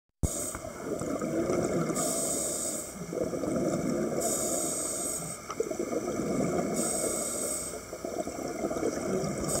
school of hammer head shark
2019年12月18日 沖縄県与那国島 ダイビングポイント「 西崎（いりざき）」
気温24℃　東南東の風 5m　波1m　透明度20ｍ以上　水温24℃

ハンマーヘッドシャーク100ほどの群れを3回 見ることができました。

3回目、最後の群れ。

撮影；与那国ダイビングサービス ゲスト：ITUさん